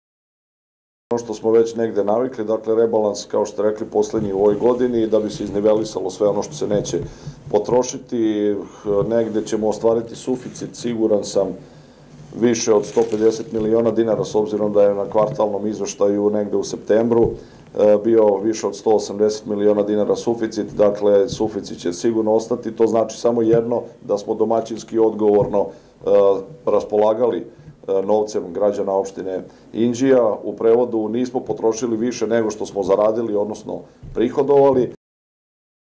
Gak: Domaćinsko poslovanje u 2022.
Opština Inđija završava ovu godinu sa suficitom u budžetu od 150 miliona dinara, rekao je predsednik Vladimir Gak komentarišući predstojeću sednicu Skupštine opštine Inđija na kojoj je dnevnim redom predviđeno donošenje Odluke o rebalansu budžeta za tekuću godinu.  To je, između ostalog potvrda da se domaćinski poslovalao tokom 2022. godine. Sednica je zakazana za četvrtak 22. decembar  sa početkom u 10 sati.